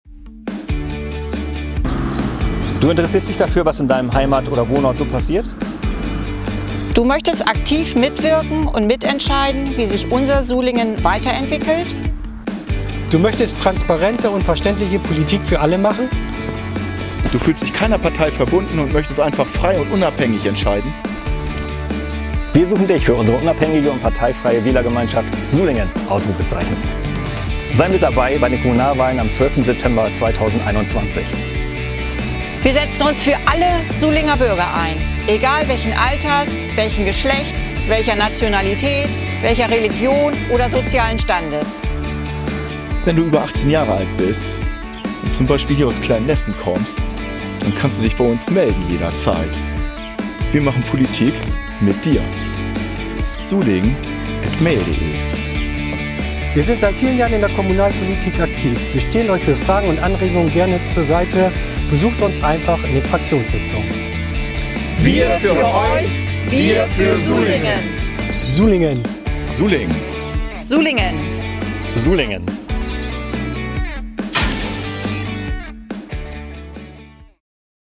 "Sulingen!" - Die unabhängige und parteifreie Wählergemeinschaft!
„Du interessierst Dich dafür, was in Deinem Heimat- bzw. Wohnort so alles passiert?“
„Du möchtest aktiv daran mitwirken und mitentscheiden wie sich Dein Ort weiterentwickelt?“
„Du fühlst Dich aber keiner Partei verbunden oder möchtest einfach frei und unabhängig entscheiden?“
„Du möchtest transparente und verständliche Politik für alle Bürger machen?“

„Dann suchen wir Dich für unsere unabhängige und parteifreie Wählergemeinschaft „Sulingen!“ mit der wir bei den nächsten Kommunalwahlen am 12. September 2021 antreten.“

„Wir setzen uns für alle Bürger Sulingens ein – egal welchen Alters, welchen Geschlechts, welcher Nationalität, Religion oder sozialen Standes!“

„Wenn Du mindestens 18 Jahre alt bist und Deinen Wohnsitz in Sulingen, Nordsulingen, Lindern, Rathlosen, Klein oder Groß Lessen hast, dann melde Dich gerne bei uns auf Facebook, Instagram oder Youtube unter der „Sulinger Ratsinfo“ oder per Email an “ “!

„Wir sind bereits seit mehreren Jahren kommunalpolitisch aktiv und stehen Euch gerne jederzeit für Fragen und Anregungen zur Verfügung. Kommt uns doch auch einfach mal bei unseren öffentlichen Fraktionssitzungen besuchen!“

„Wir für Euch! – Wir für Sulingen!“